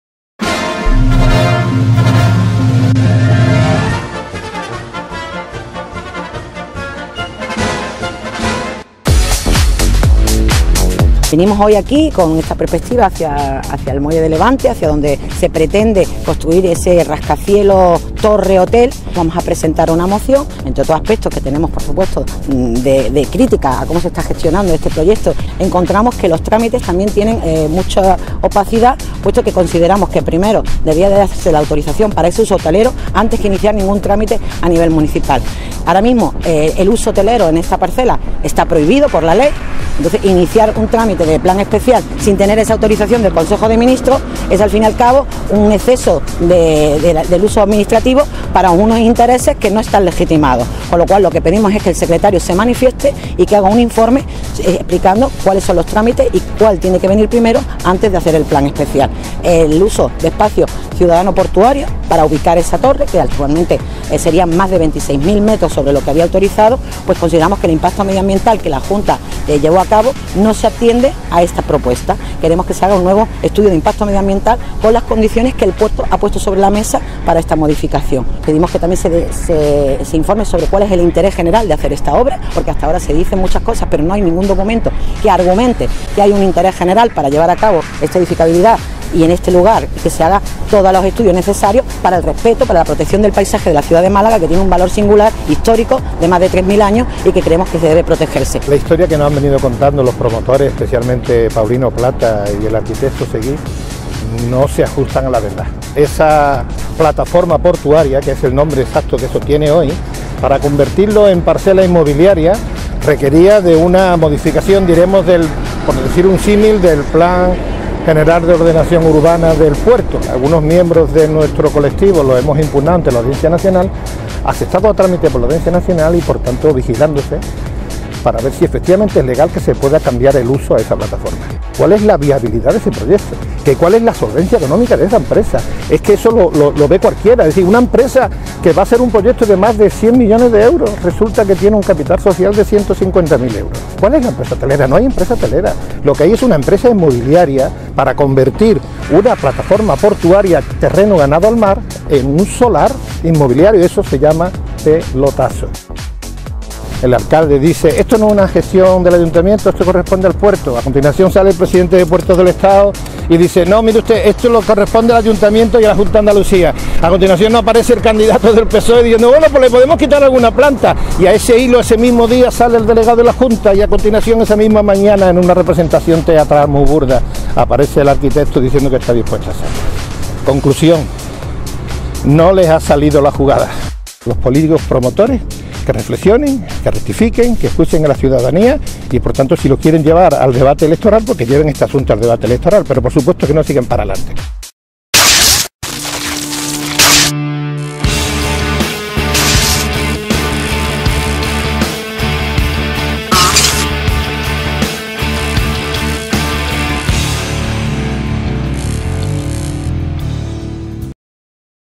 El Ayuntamiento no ha justificado el interés general del hotel-rascacielos del puerto
Málaga Ahora pide en la Comisión de Urbanismo que se aclaren los trámites para permitir el uso hotelero en dominio público, lo que actualmente no permite la ley, y un estudio municipal de impacto antes de cualquier modificación del Plan Especial.